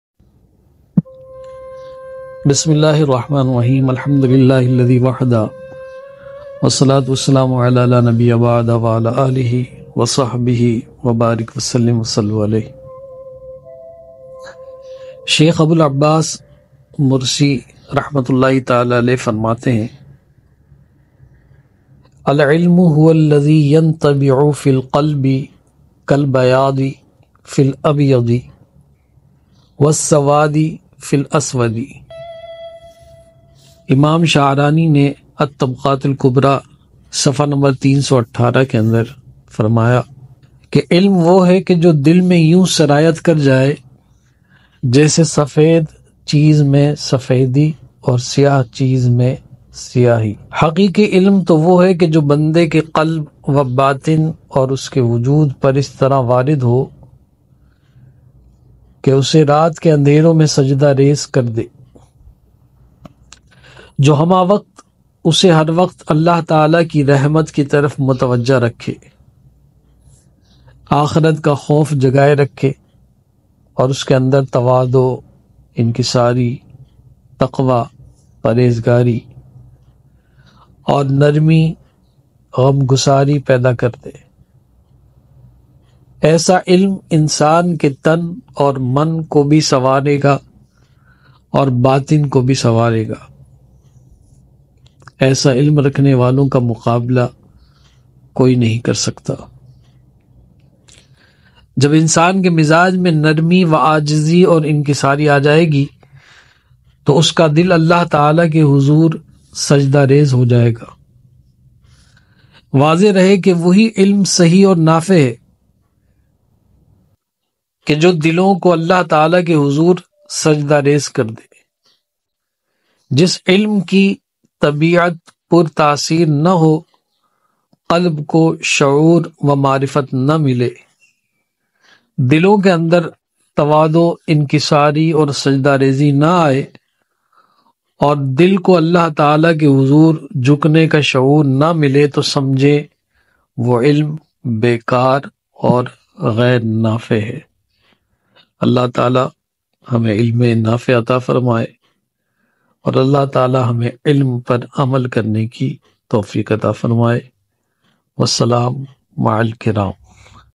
Sufi Aqwal || ilme Haqeeqi || (قول : حضرت ابوالعباس مرسی رح) || voice status ♥️
شیخ ابو العباس مُرسی فرماتے ہیں:

اَلْعِلْمُ هُوَ الَّذِي یَنْطَبِعُ فِي الْقَلْبِ کَالْبَیَاضِ فِي الْأَبْیَضِ، وَالسَّوَادِ فِي الْأَسْوَدِ.

شعرانی، الطبقات الکبری: 318
علم وہ ہے جو دل میں یوں سرایت کر جائے جیسے سفید چیز میں سفیدی اور سیاہ چیز میں سیاہی۔

حقیقی علم تو وہ ہے جو بندے کے قلب و باطن اور اُس کے وجود پر اِس طرح وارد ہو کہ اسے رات کے اندھیروں میں سجدہ ریز کر دے

جو ہمہ وقت اسے ہر وقت اللہ تعالیٰ کی رحمت کی طرف متوجہ رکھے ، آخرت کا خوف جگائے رکھے اور اس کے اندر تواضع و انکساری، تقویٰ و پرہیزگاری اور نرمی و غم گساری پیدا کر دے۔ ایسا علم انسان کے تن اور من کو بھی سنوارے گا اور باطن کو بھی نکھار دے گا۔ ایسا علم رکھنے والوں کا مقابلہ کوئی نہیں کر سکتا۔

جب اِنسان کے مزاج میں نرمی و عاجزی اور اِنکساری آجائے گی تو اس کا دل اللہ تعالیٰ کے حضور سجدہ ریز ہو جائے گا۔ واضح رہے کہ وہی علم صحیح اور نافع ہے جو دلوں کو اللہ تعالیٰ کے حضور سجدہ ریز کر دے۔ جس علم کی طبیعت پُرتاثیر نہ ہو، قلب کو شعور و معرفت نہ ملے، دلوں کے اندر تواضع، انکساری اور سجدہ ریزی نہ آئے اور دل کو اللہ تعالیٰ کے حضور جھکنے کا شعور نہ ملے تو سمجھیں وہ علم بے کار اور غیر نافع ہے،